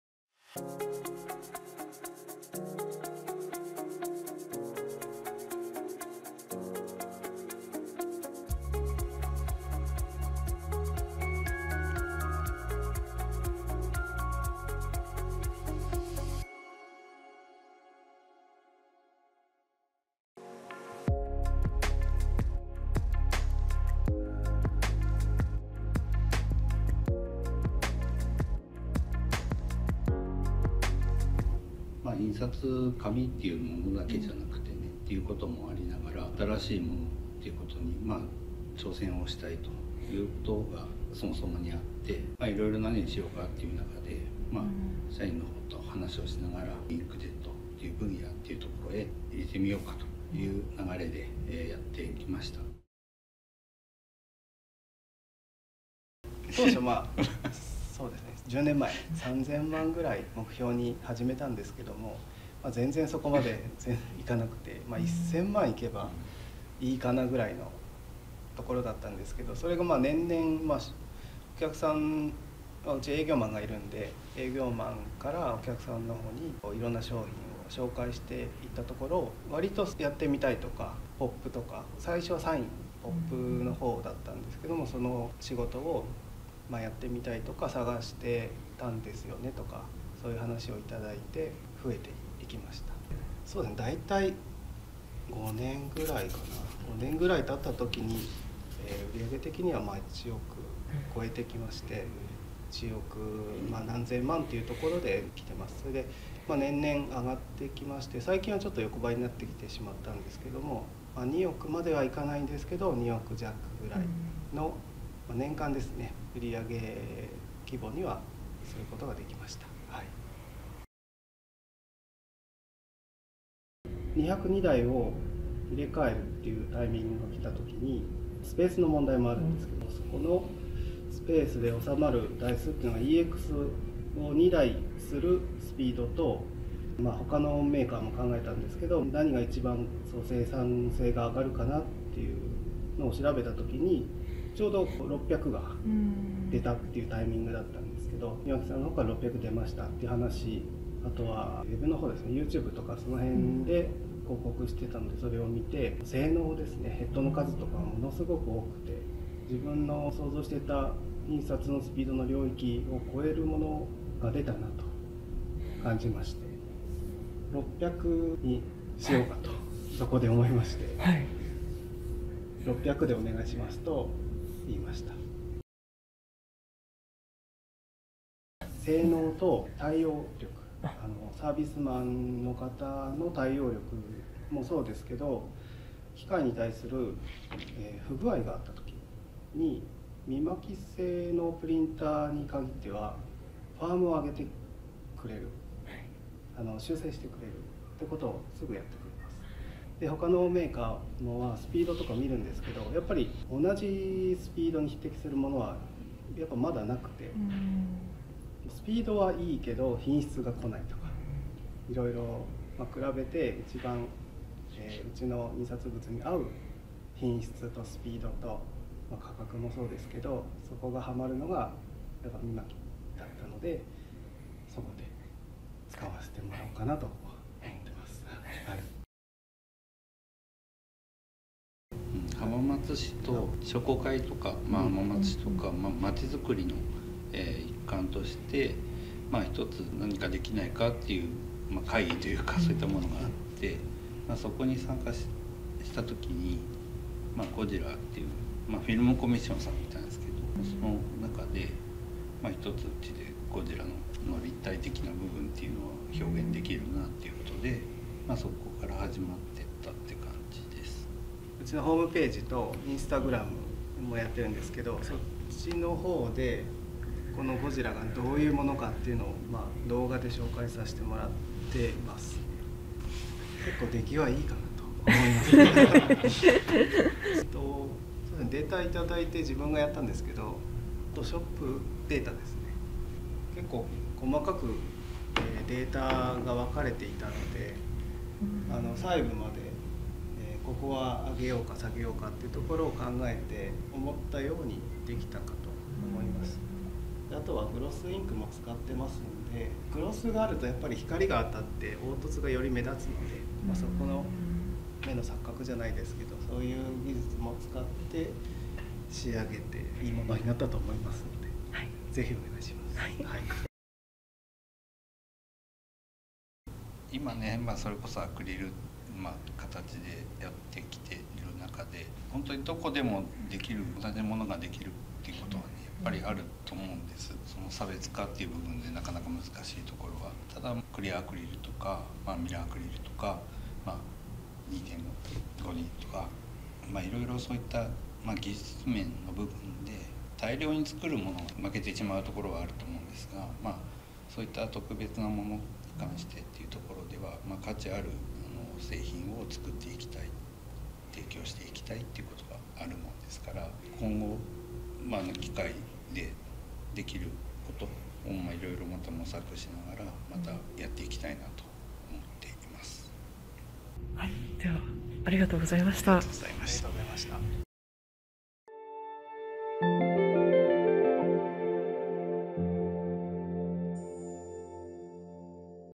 【製品導入事例】JFX600-2513｜松本印刷株式会社様
大判フラットベッドUV-LED方式インクジェットプリンタ「JFX600-2513」
想像を超える生産性の高さが導入の決め手

動画内でも紹介されている、「ゴジラ-1.0」の「アートフルパネル2.5D」は、下記にて販売されております。
こちらもJFX600-2513にて製作されています。
（松本印刷株式会社様が運営する一作屋のページにアクセスします。）